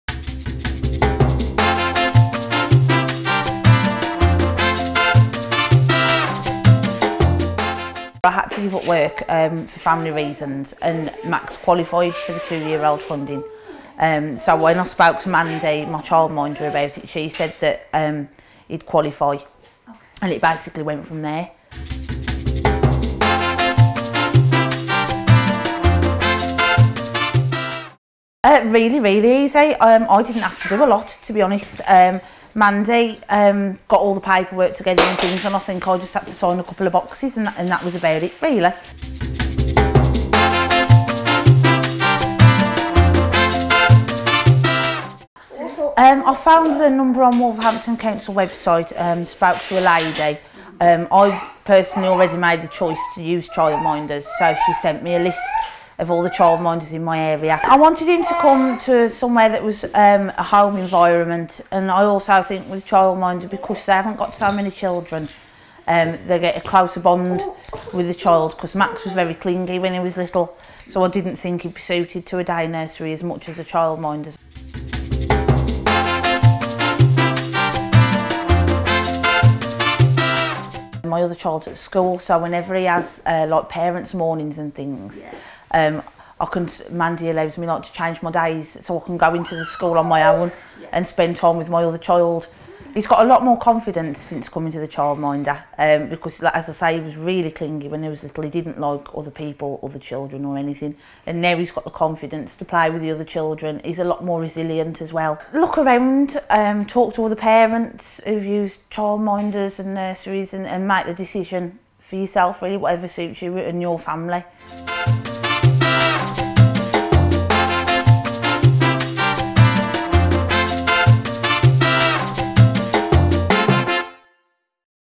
Find out why this parent chose a child minding setting for her son using Terrific for Twos funding.
Terrific for Twos funding could offer your child up to 15 hours free childcare. Find out if you could apply on our